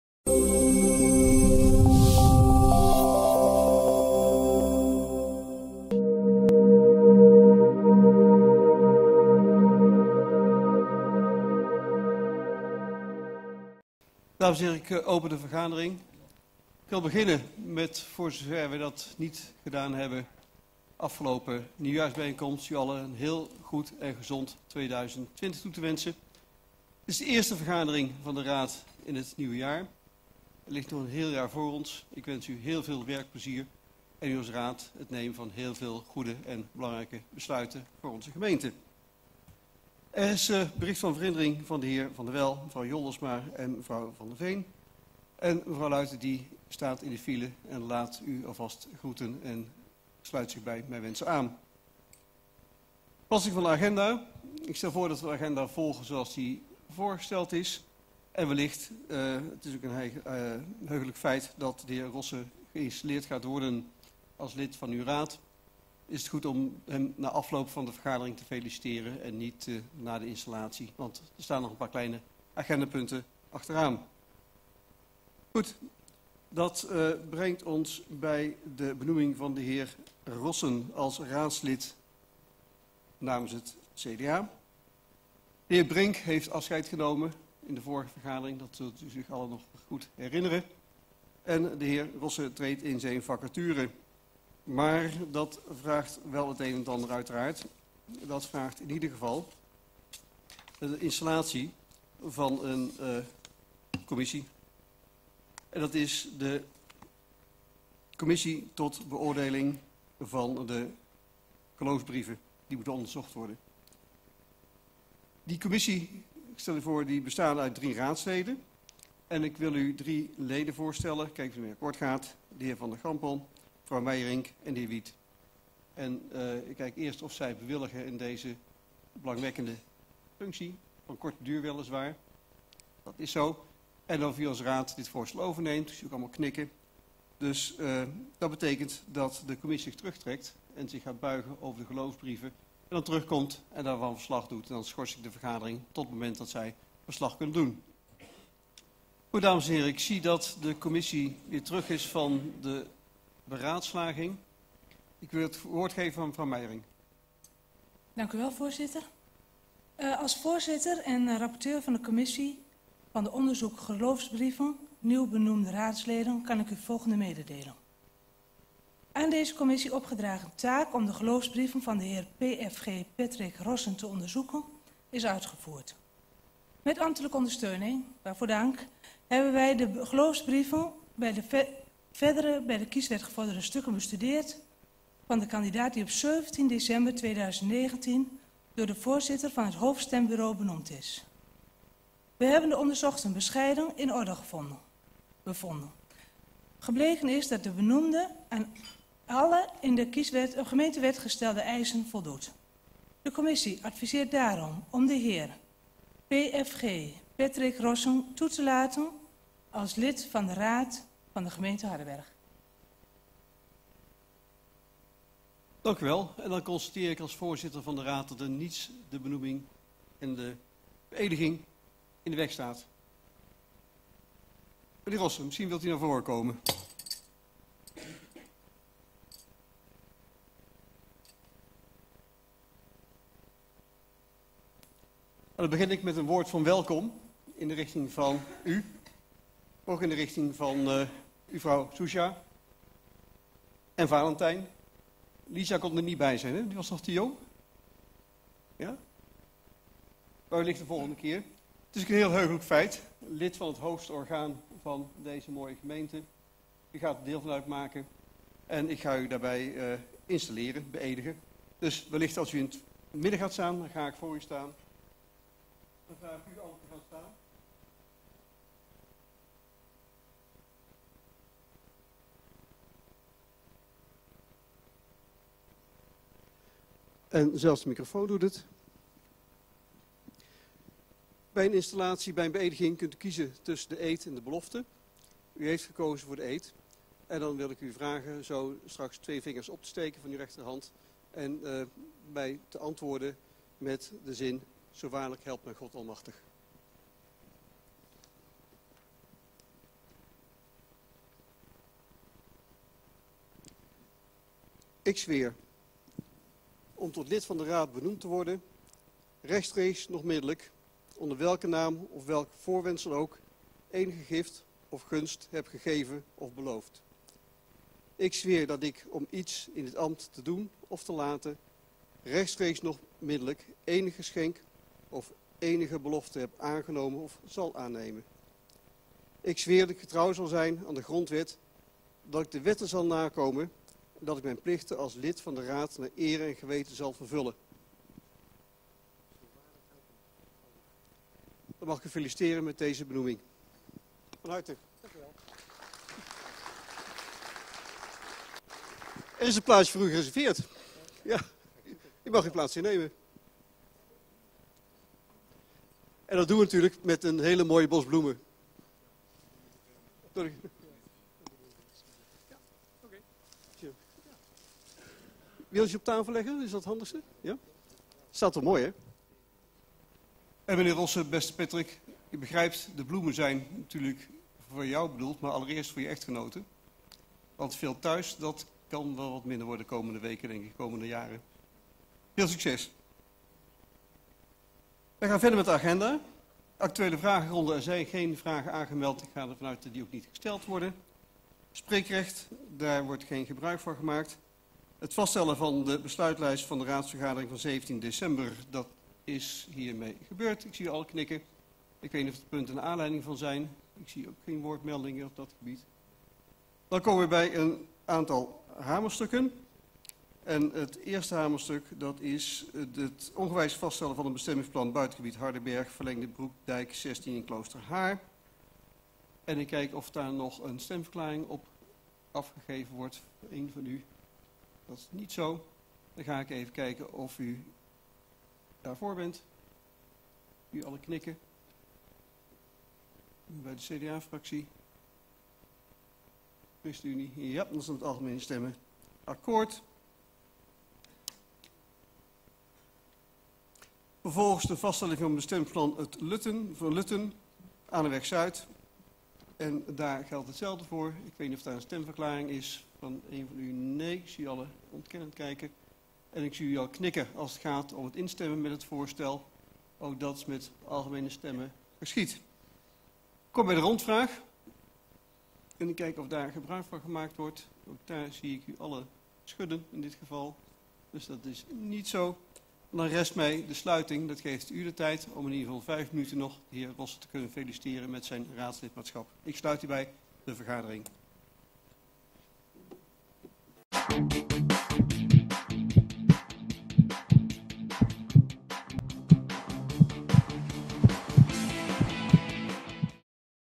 Hardenberg: Gemeenteraad van 14 januari 2020
de heer drs. J.W. Wiggers
Agenda
1. Opening en mededelingen
2. Vaststelling agenda
3. Benoeming de heer P.F.G. Rossen als raadslid namens CDA
4. Actuele Vragenronde
5. Spreekrecht agendapunten
6. Vaststellen besluitenlijst raadsvergadering d.d. 17 december 2019
 Hamerstukken (alleen stemverklaring mogelijk)
7. Het ongewijzigd vaststellen van het bestemmingsplan 'Buitengebied Hardenberg, Verlengde Broekdijk 16 Kloosterhaar' voor het omzetten van een bedrijfsbestemming naar een dagrecreatieve bestemming ten behoeve van een kleinschalig streekmuseum en het omzetten van een bedrijfsbestemming naar de bestemming 'bos'
8. Vaststelling bestemmingsplan 'Lutten, Anerweg-Zuid naast nr. 9 Lutten'
9. Rondvraag
10. Sluiting
en via Internet op alle apparaten op of via Instagram.